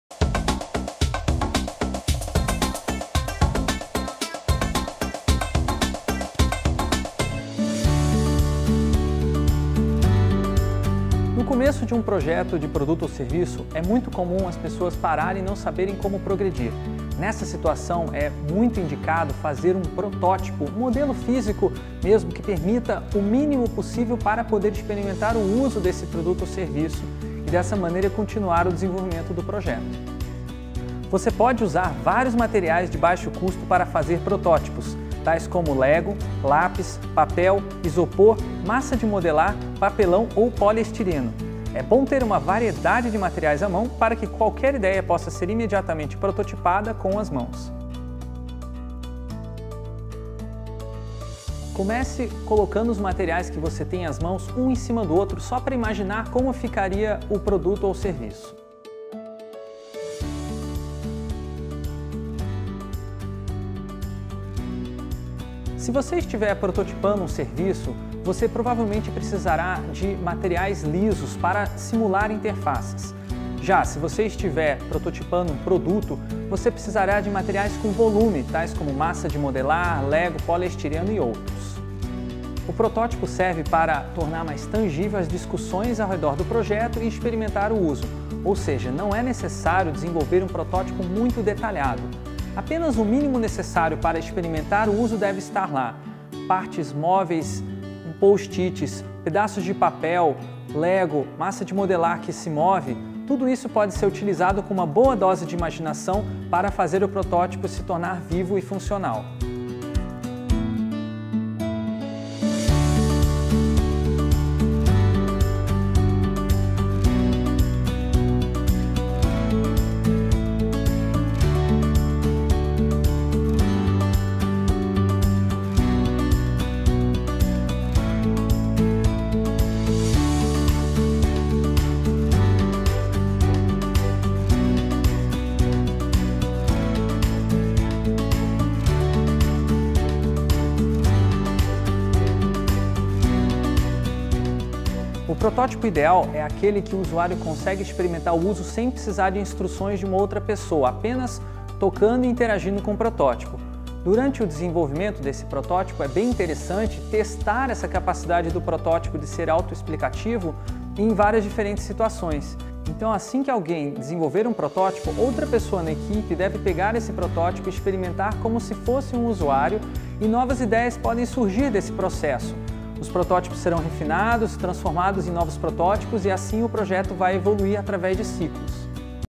Como fazer um protótipo bruto (ENG Subs)
Protótipo bruto é um modelo cru do produto ou serviço, exibindo apenas o necessário para simular o uso. O modelo pode ser feito com lápis e papel, isopor, massa de modelar, Lego, papelão ou qualquer outro material de baixo custo.

Por que é feito?

Um conceito que funciona muito bem na ideia pode não funcionar no mundo físico. Ao construir o protótipo, a ideia costuma ser adaptada para viabilizar sua execução. O protótipo ajuda a refletir sobre a ideia e torná-la mais concreta.

Como é feito?

1) Conseguir os materiais que parecem ser necessários para criar o protótipo.

2) Experimentar combinações dos vários materiais para dar forma ao protótipo. Tirar foto ou captura de tela de cada uma.

3) Implementar as funcionalidades do protótipo através de partes móveis, desenhos, texto de instrução e outras marcações.

4) Imaginar diversos usos do protótipo falando em voz alta o que você está fazendo.

5) Dentre os experimentos feitos, escolher um protótipo para testar na próxima fase do projeto.

Créditos: Vídeo produzido pela Hotmilk PUCPR e Imago Produções Educativas em 2017, como material didático para o programa de inovação aberta Renault Experience. Esta cópia faz parte do arquivo pessoal de Frederick van Amstel, responsável pelo roteiro e apresentação. Direção: Tatiana de Paula Lopes.